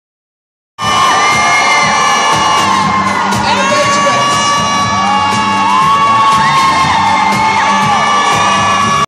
Bx P.R. Day Parade Men's Pagean4 2014, pt4
May 17, 2014. The Bronx Puerto Rican Day Parade Men's Pageant, pt 4 (note: I ran out of memory so it's only 9 sec)